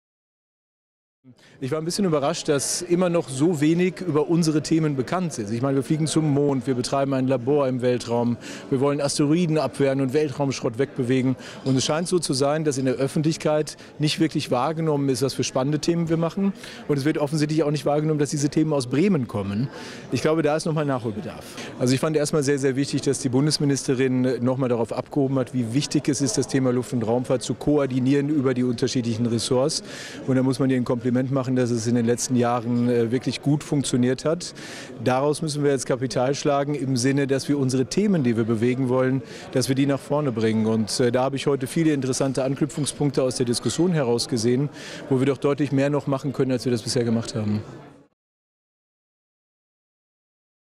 Dr. Juckenhöfels Fazit
Bremen, 09.06.17: Bremen ist Raumfahrtstadt. Grund genug für den WESER-KURIER hochrangige Gäste zur wkkonferenz einzuladen. Dr. Oliver Juckenhöfel leitet den Bremer Airbus-Standort und bringt nach heute neue Erkenntnisse mit in seine Firma.